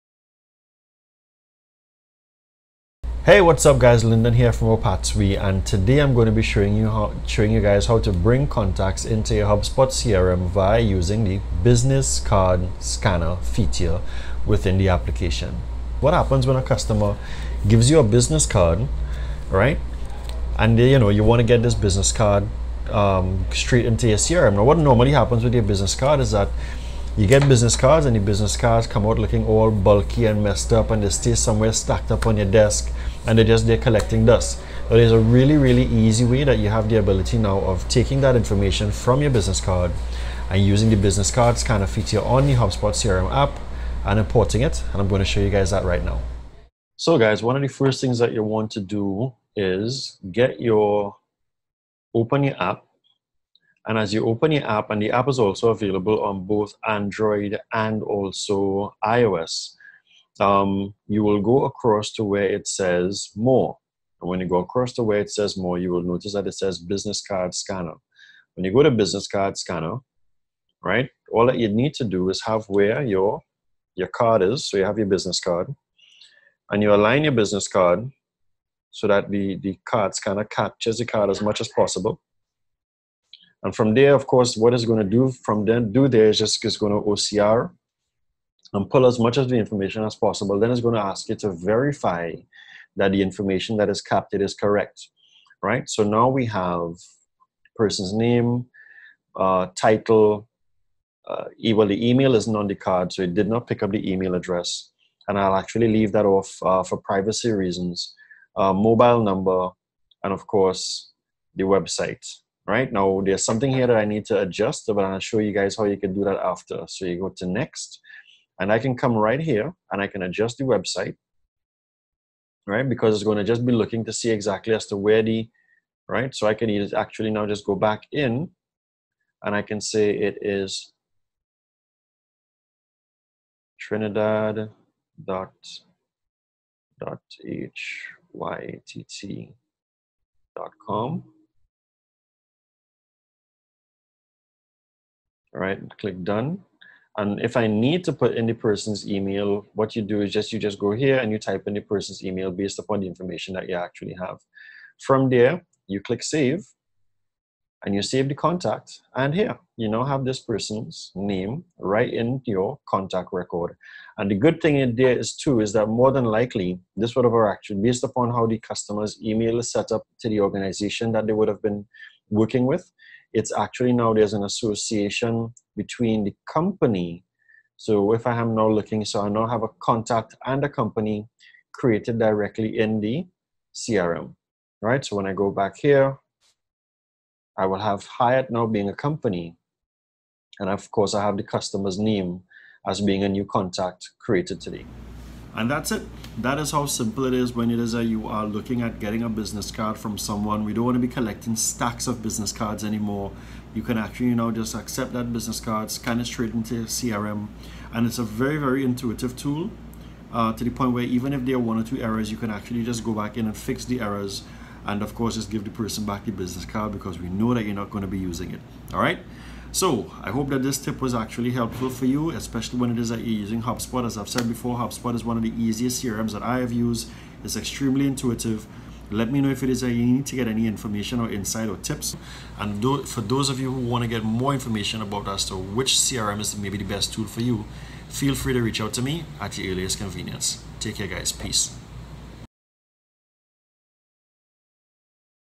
HubSpot In-App Business Card Scanner Feature
With everyone on the go these days we tend to find that if someone gives us a business card and we don’t take that information down immediately it will get lost somewhere, believe us when we say that 🤦🏾‍♂️.

Or let’s say you do write down the info or store in your phone immediately what do you do with the business card next? Ohh that’s right, store it in the drawer with the other business cards 🙂. As much as we make fun of this guys, it’s a reality that we either lose the info or the business card stack becomes a mountain.

In this video, we share a pretty cool in-App business card scanner feature from Hubspot that makes life easier and more productive.